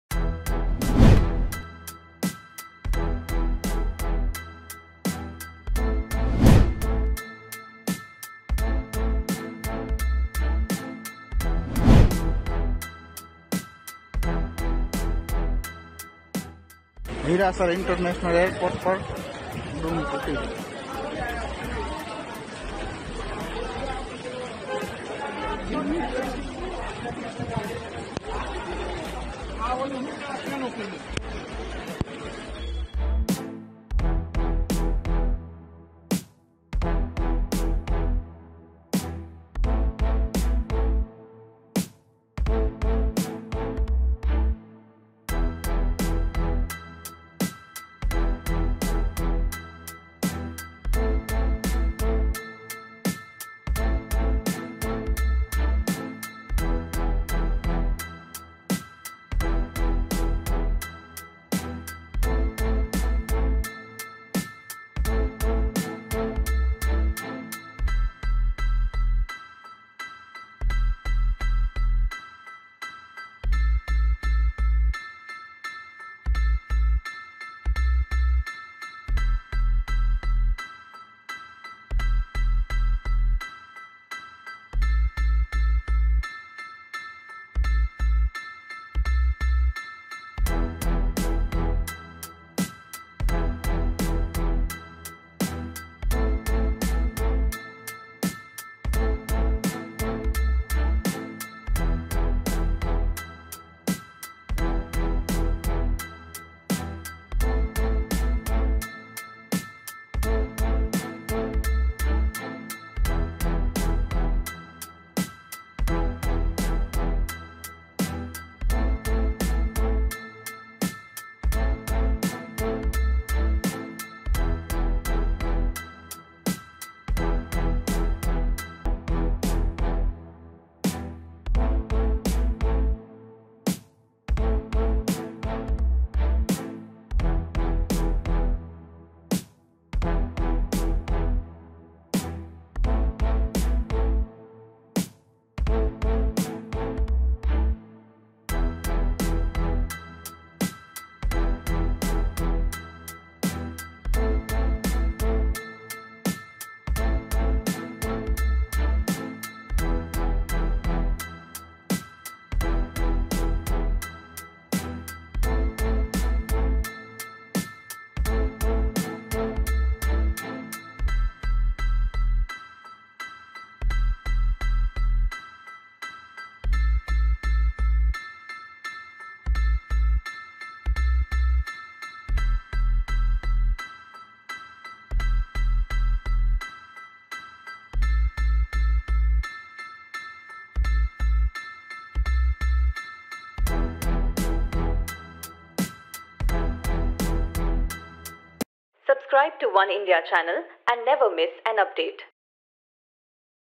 Rajkot Airport Roof Canopy Collapses After Heavy Rain, No Injuries Reported
Less than a year after Prime Minister Narendra Modi inaugurated the Rajkot International Airport (RIA) at Hirasar village, a section of the roof canopy in the terminal’s passenger drop and pickup area collapsed following heavy rain on Saturday. Fortunately, no injuries were reported and airport operations were not affected. This incident follows a similar roof collapse at Delhi's Indira Gandhi International Airport after record rainfall. The temporary terminal, still under construction, experienced a canopy failure due to water accumulation. RIA officials are conducting an inquiry and will submit a detailed report to the Airports Authority of India (AAI).